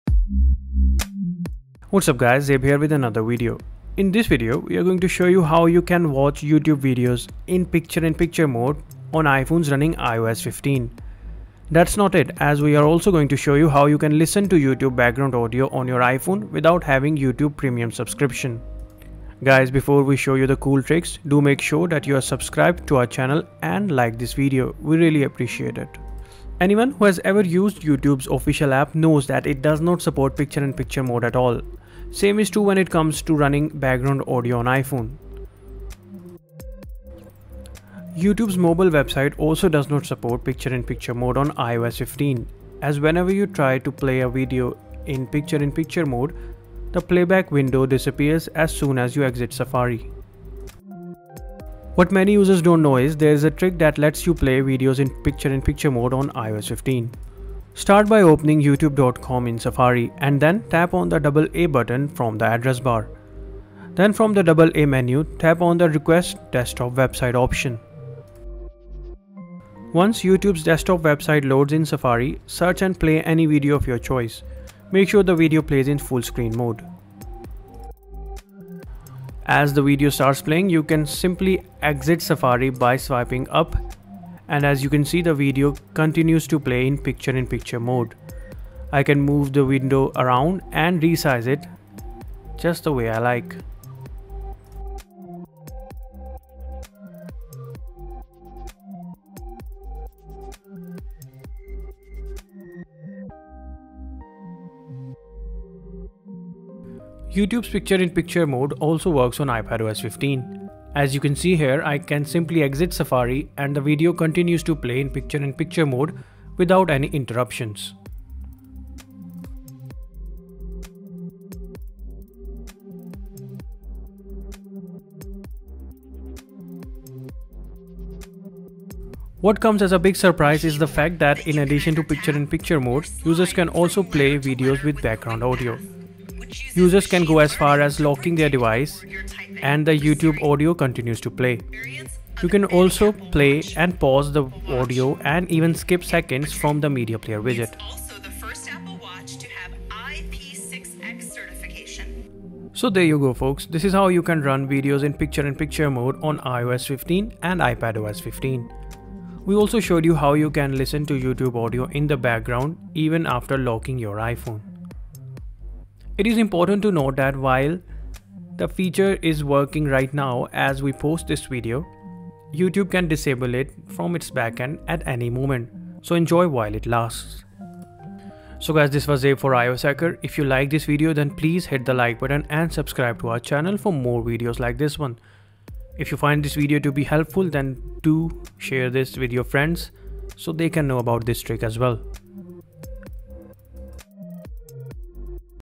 Play YouTube Videos In iOS 15 Picture-in-Picture & YouTube Background Audio iPhone Trick
In this video we show you how you can play YouTube videos in iOS 15's Picture in Picture mode without YouTube Premium. This also works on iPadOS 15. YouTube has blocked Picture In Picture mode in its iOS app and mobile website, however in this trick we show you how to play videos in PiP mode on iPhone by accessing its desktop website.

The video also shows you how to play YouTube background audio on iPhone without YouTube Premium. The best thing about the tricks featured in this video is that you do not need any third-party app or rely on Siri Shortcut, which makes the process easy and convenient.